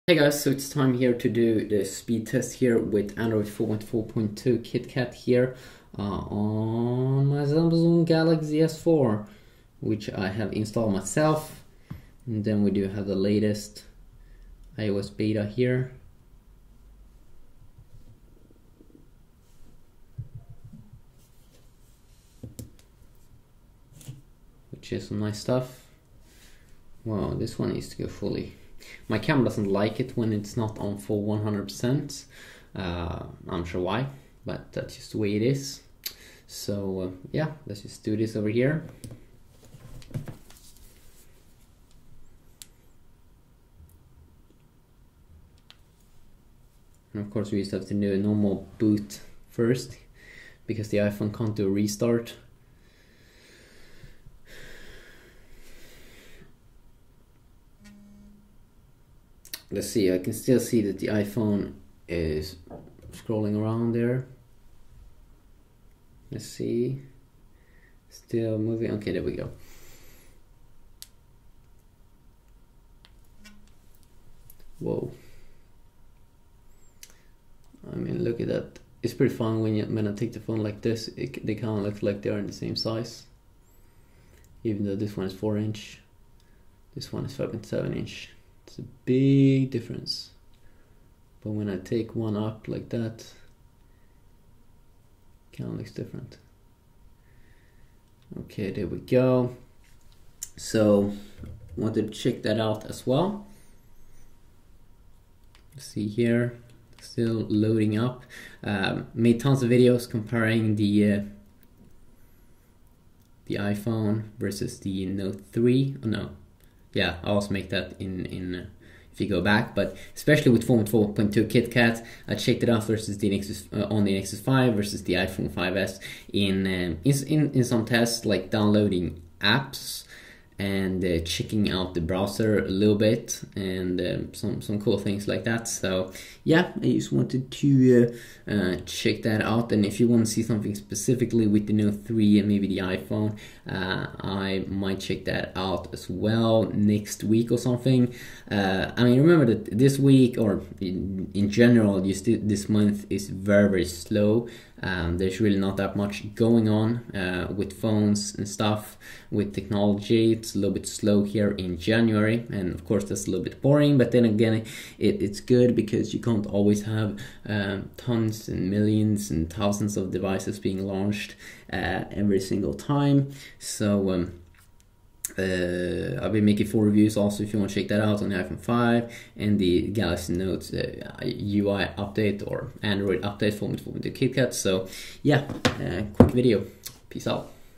Apple iPhone 5S iOS 7.1 Beta 4 vs. Samsung Galaxy Note 3 Android 4.4 - Which Is Faster?
Hi, and welcome to my tech channel. My name is Adrian and I'm a swedish YouTuber.